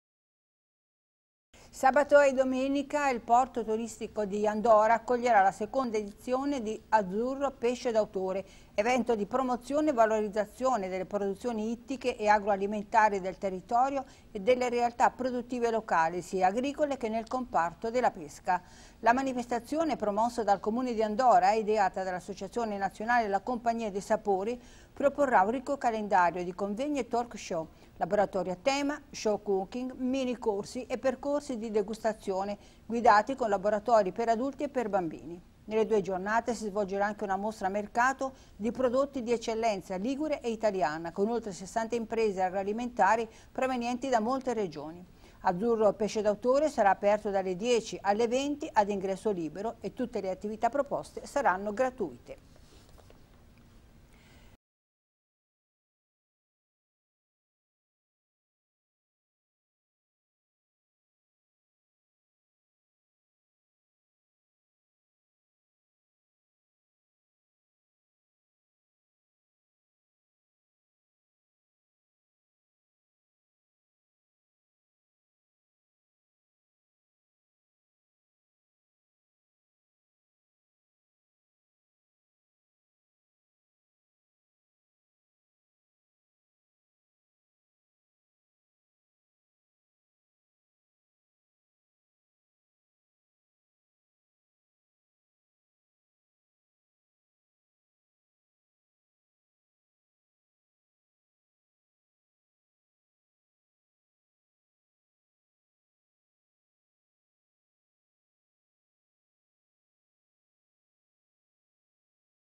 ANDORA : AZZURRO PESCE D' AUTORE
Andora. Sabato e domenica il porto turistico di Andora accoglie la seconda edizione di “Azzurro, pesce d’ autore”, evento di promozione e valorizzazione delle produzioni ittiche e agroalimentari del territorio e delle realtà produttive locali, sia agricole che del comparto della pesca.
La manifestazione, promossa dal Comune di Andora e ideata dall’Associazione Nazionale La Compagnia dei Sapori con il contributo di Totem Eventi, si fregia del marchio Anci per Expo e propone un ricco calendario di convegni e talk show, laboratori a tema, show cooking, mini corsi e percorsi di degustazione guidati, laboratori per adulti e bambini. Nelle due giornate si svolgerà anche una mostra mercato di prodotti di eccellenza ligure ed italiana, con oltre 60 imprese agroalimentari provenienti da molte regioni. Ben 11 le postazioni dedicate allo Street food. “Azzurro, pesce d’autore” è aperto dalle 10 alle 20, è ad ingresso libero e tutte le attività proposte sono gratuite